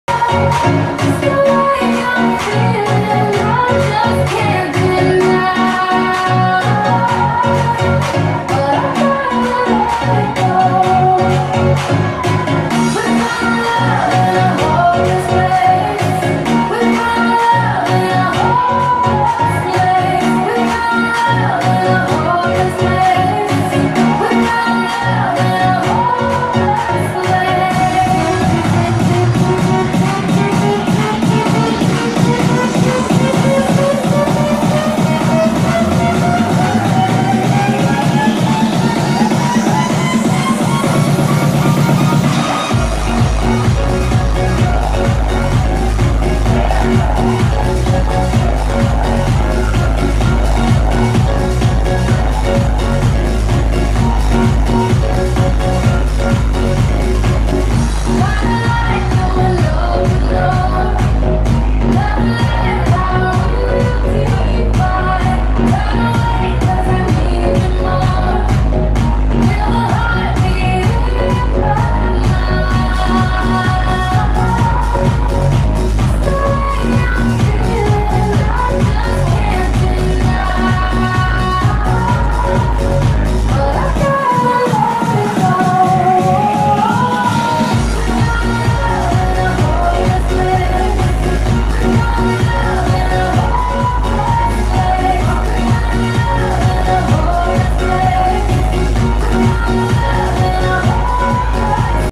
Pink punters Zoe dancing cocktail dress and 6 inch heels 360 VIDEO
Me dancing with sexy lovely guy at pink punters last Friday.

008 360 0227 dance with man part 2 Render HD